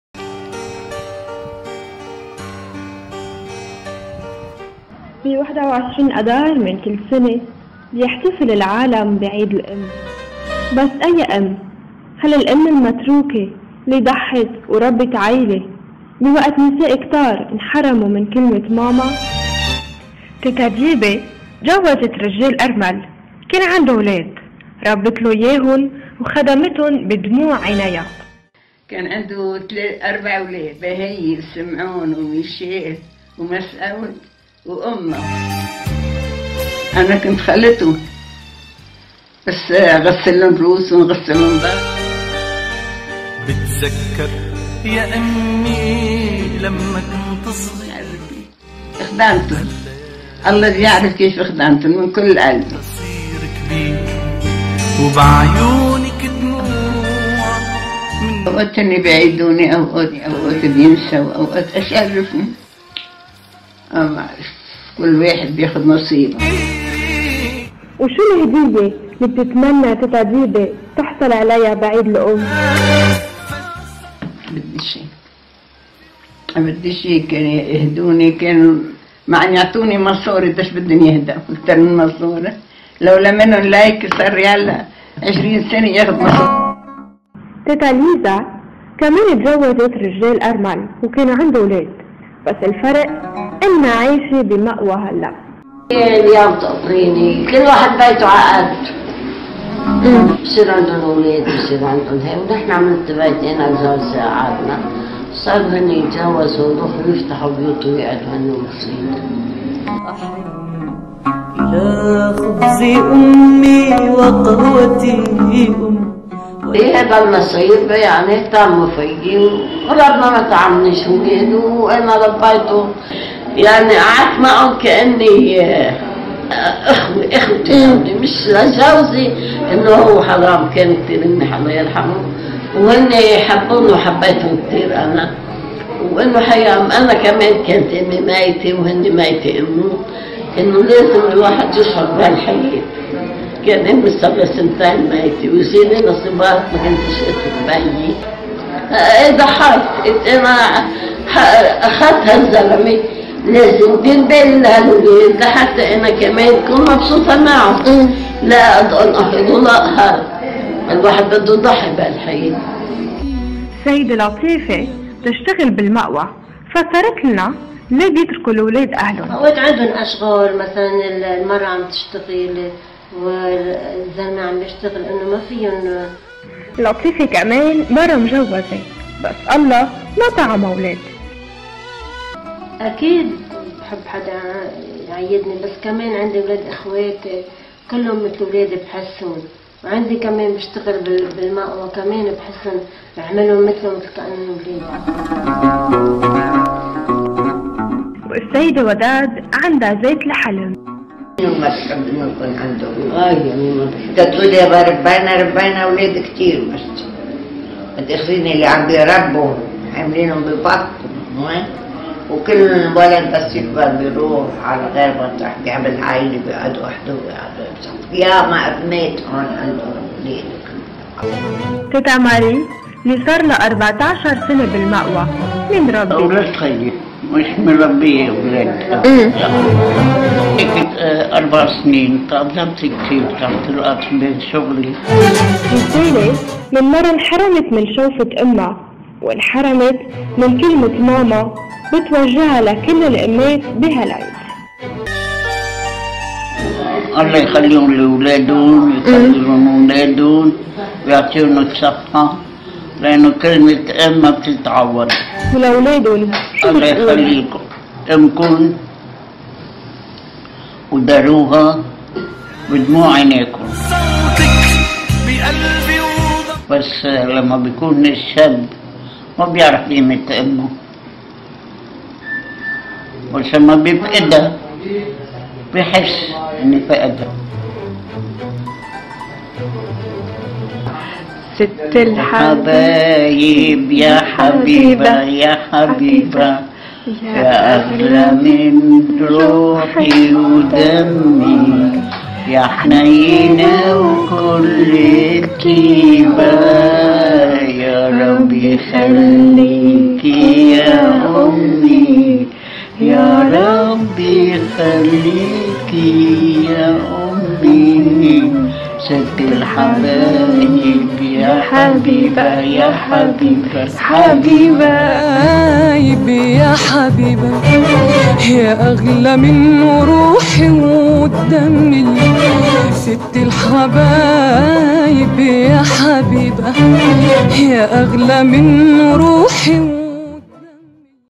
"كرمالك يا ماما" - NOW
لمناسبة عيد الأم، زار موقع "NOW" أُمهات تُركوا دون أي مُعيل، كما زار نساء حُرمن من نعمة الإنجاب، وعاد بالتقرير المصوّر التالي